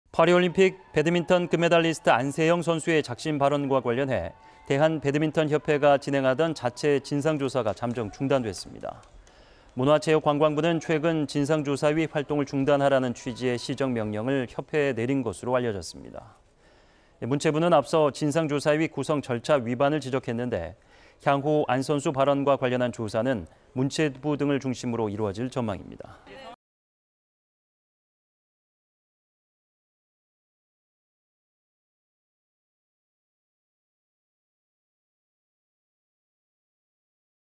배드민턴협회 '안세영 진상조사위' 문체부 시정명령으로 잠정 중단
파리올림픽 배드민턴 금메달리스트 안세영 선수의 작심 발언과 관련해 대한배드민턴협회가 진행하던 자체 진상조사가 잠정 중단됐습니다.

문화체육관광부는 최근 진상조사위 활동을 중단하라는 취지의 시정명령을 협회에 내린 것으로 알려졌습니다.

문체부는 앞서 진상조사위 구성 절차 위반을 지적했는데, 향후 안 선수 발언과 관련한 조사는 문체부 등을 중심으로 이뤄질 전망입니다.